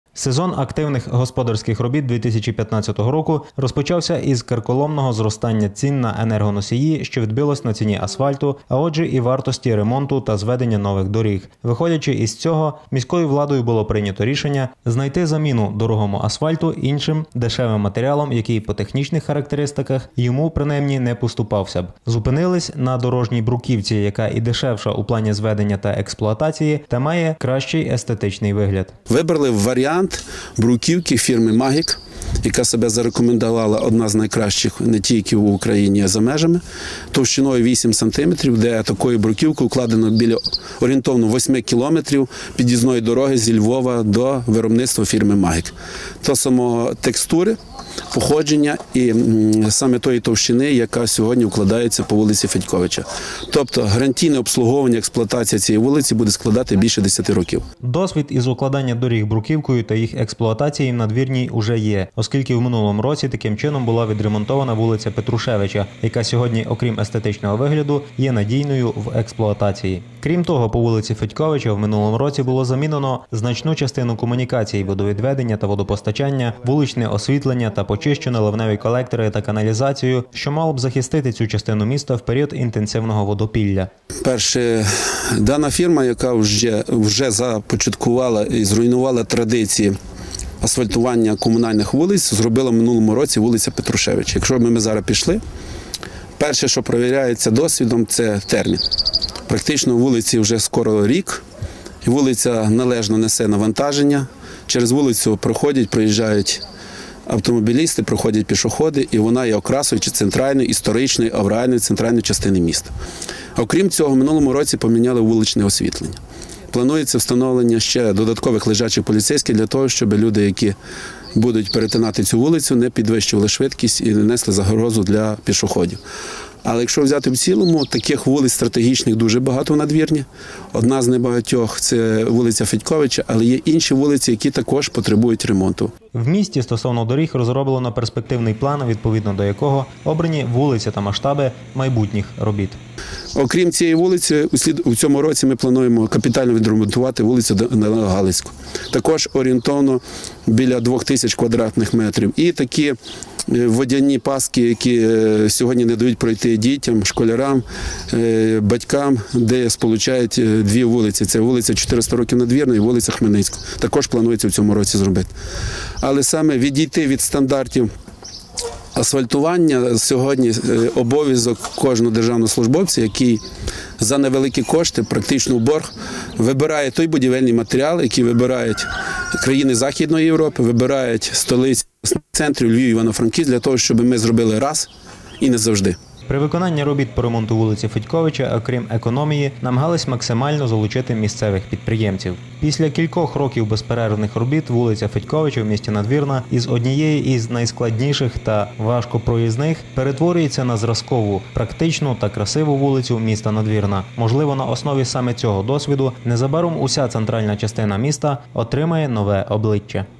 Достойні дороги для надвірнянців (12.06.2015р.)
Одна з найпроблемніших доріг міста Надвірна – вулиця Федьковича отримує нове обличчя – дорожнє полотно, встелене бруківкою. Про нові тенденції у зведенні доріг міста та оновлену вулицю Федьковича - далі у сюжеті.